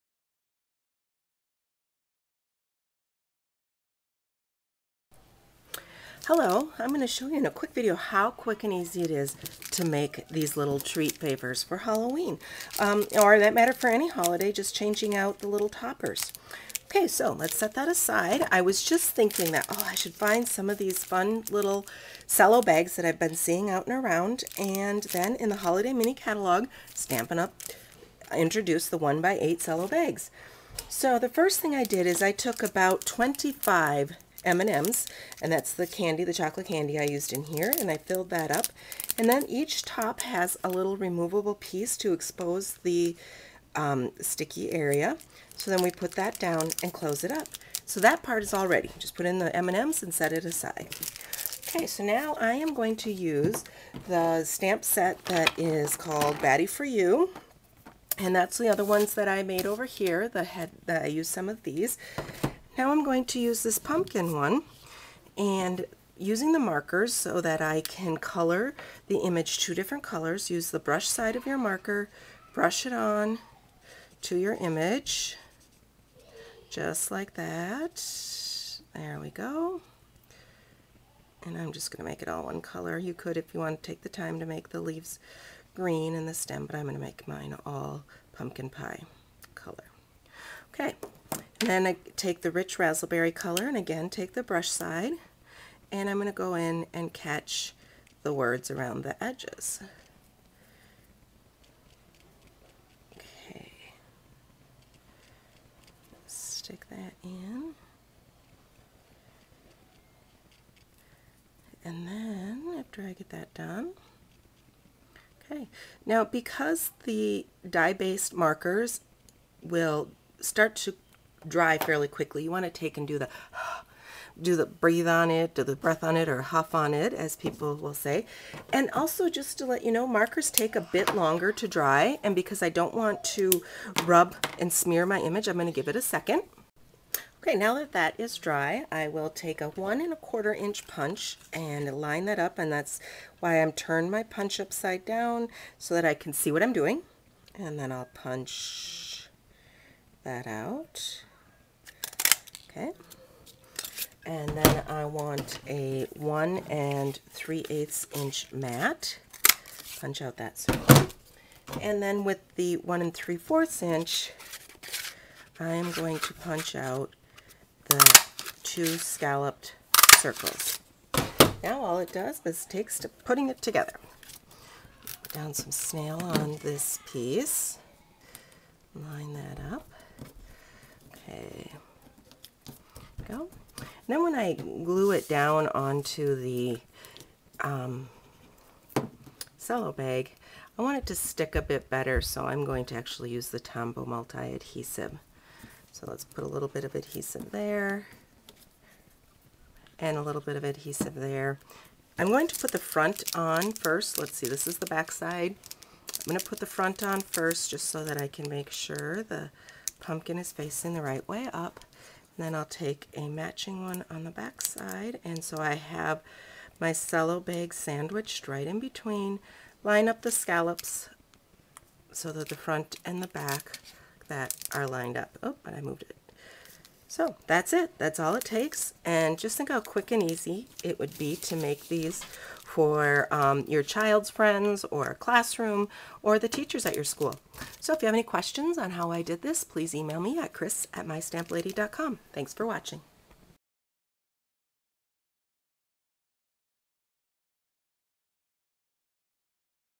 Cellophane Bag Halloween Treats
In this video you will see a very quick and easy way to make really cute Halloween treat packages.